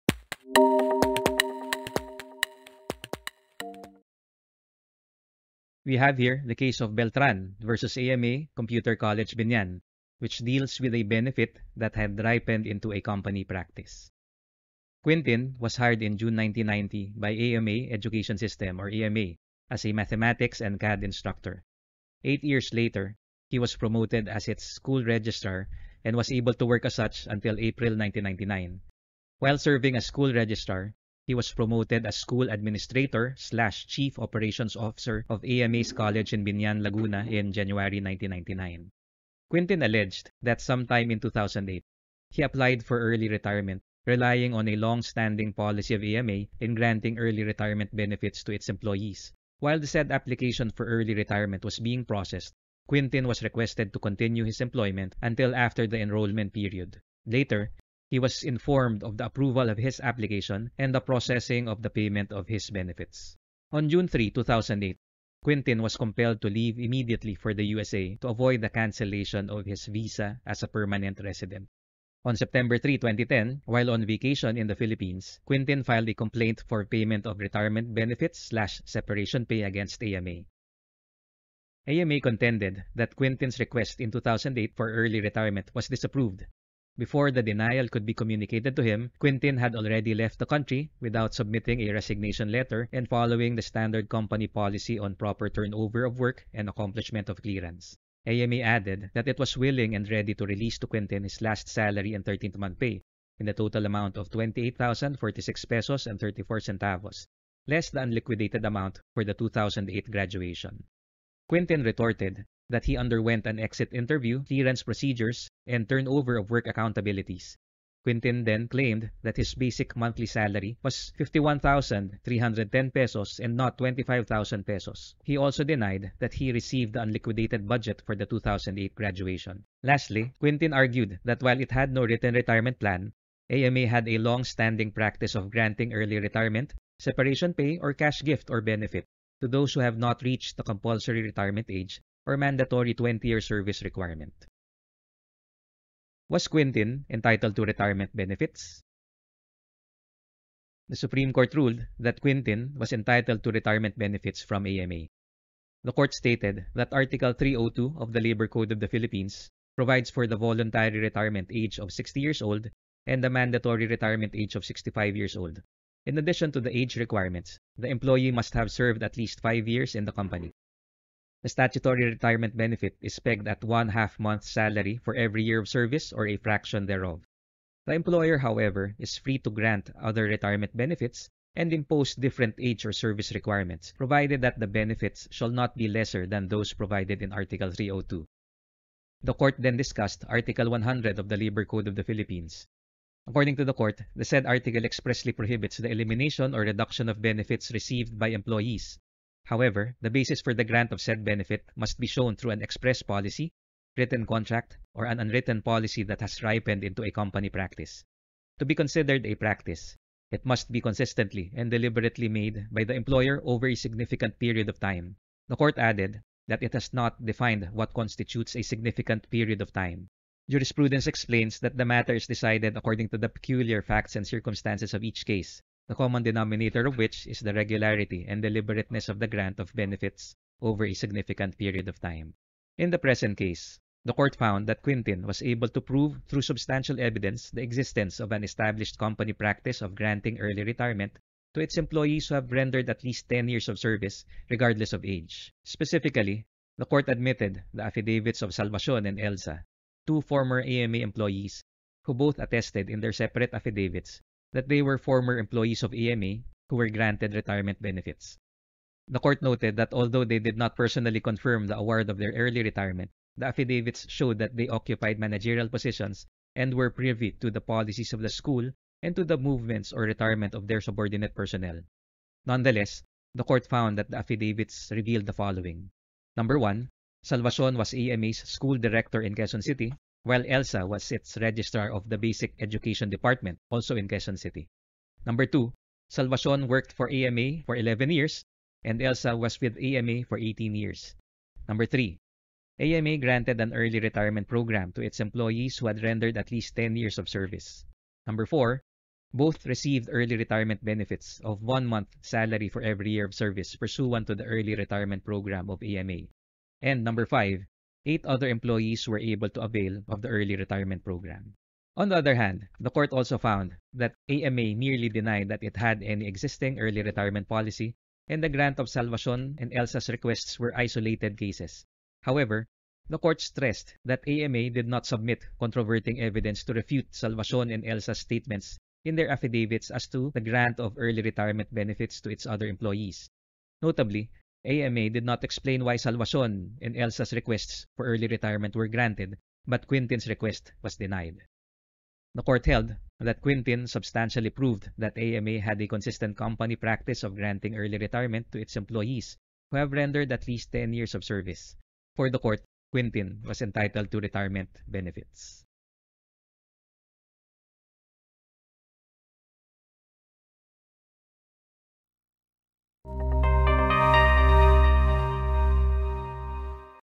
A Long-Standing Practice
Find out how this employee successfully claimed retirement benefits despite the employer’s lack of a written retirement plan.

Beltran v. AMA Computer College-Biñan, G.R. No. 223795, April 3, 2019.